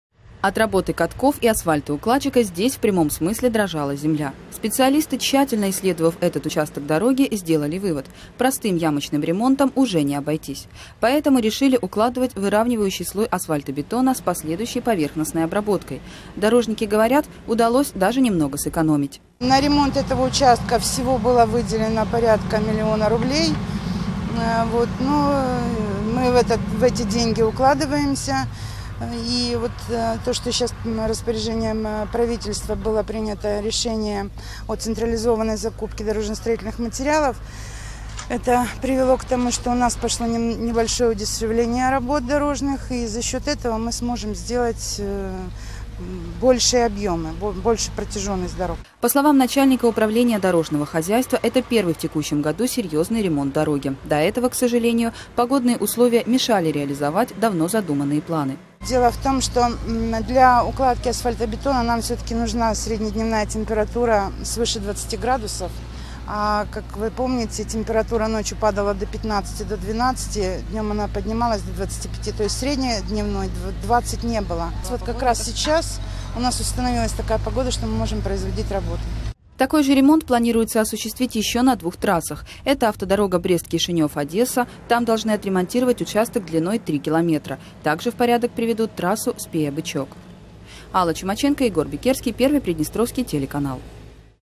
На участке трассы Тирасполь-Каменка длиной 800 метров отремонтировали дорожное покрытие
На участке трассы Тирасполь-Каменка длиной 800 метров отремонтировали дорожное покрытие. Работы проходили три дня.